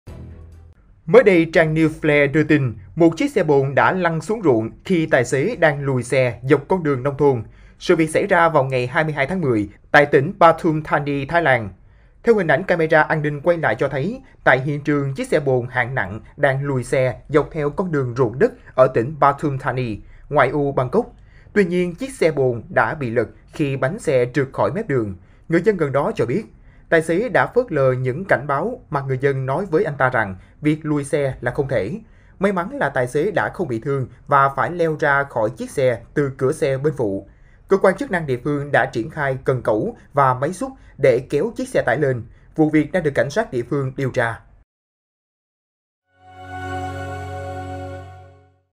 Tài xế phớt lờ cảnh báo, xe bồn 'lên bờ xuống ruộng'
Mới đây trang Newsflare đưa tin, một chiếc xe bồn đã lật xuống ruộng khi tài xế cố lùi xe dọc con đường nông thôn mà trước đó người dân đã cảnh báo là không an toàn. Sự việc xảy ra vào ngày 22-10, tại tỉnh Pathum Thani, Thái Lan.